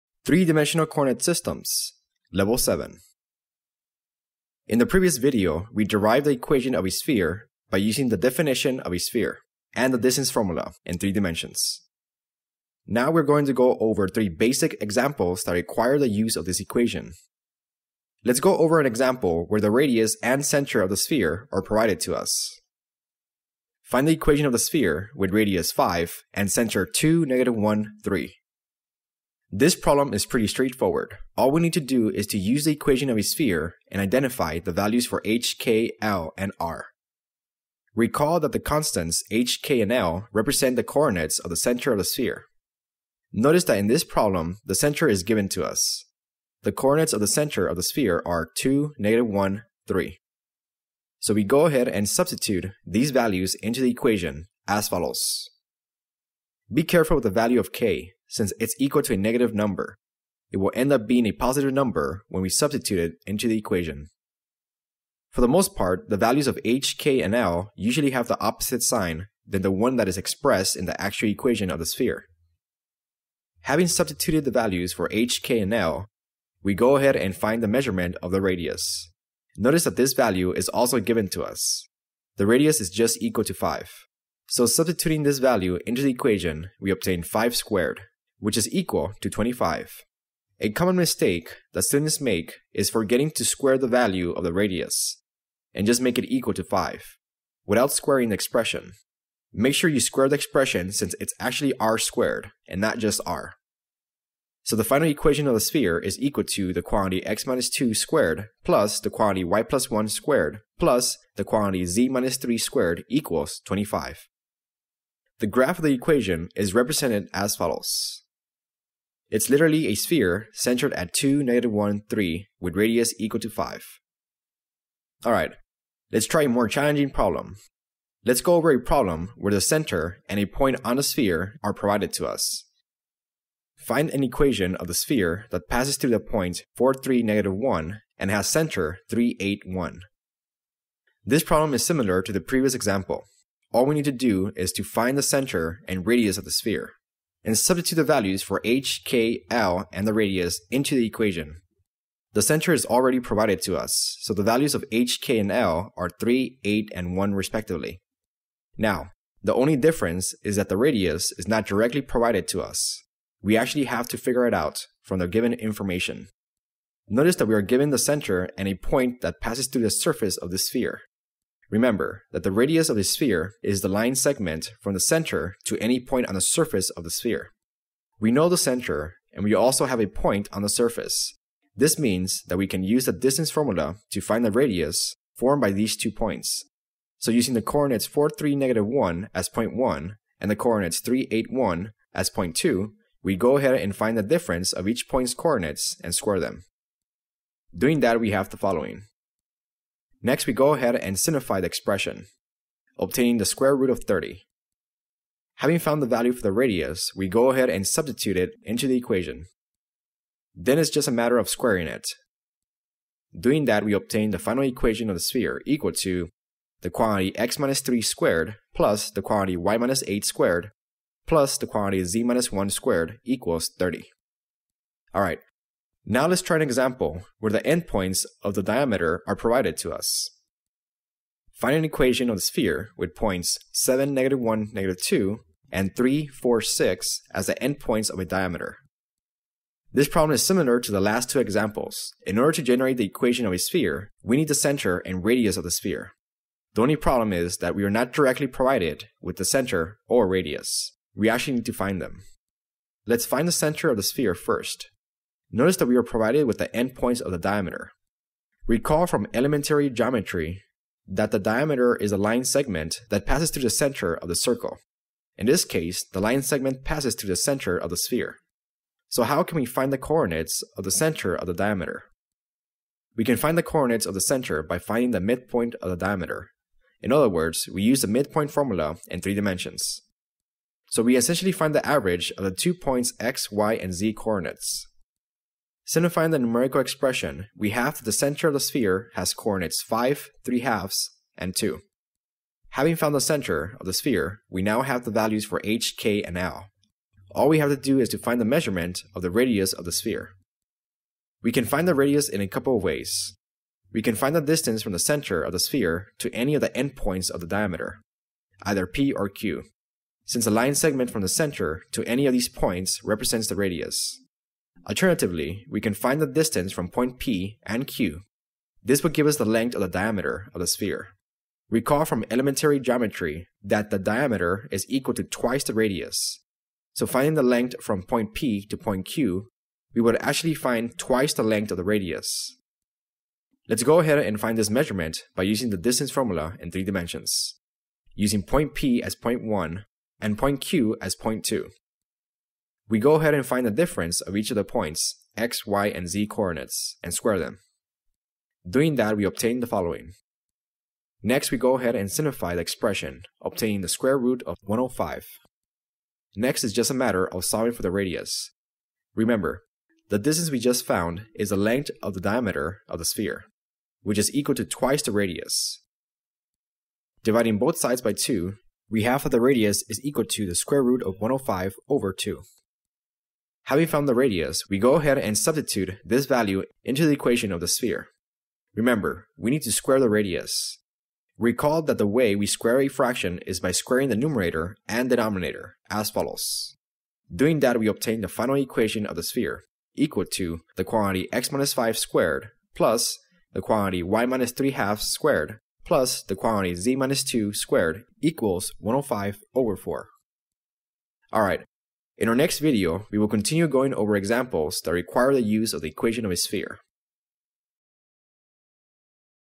Basic Sphere Examples
This video continues the exploration of a three dimensional cartesian coordinate system. This video covers three basic examples that requires the use of the equation of a sphere.

Topics:
0:20 Example 1
2:10 Example 2
3:47 Example 3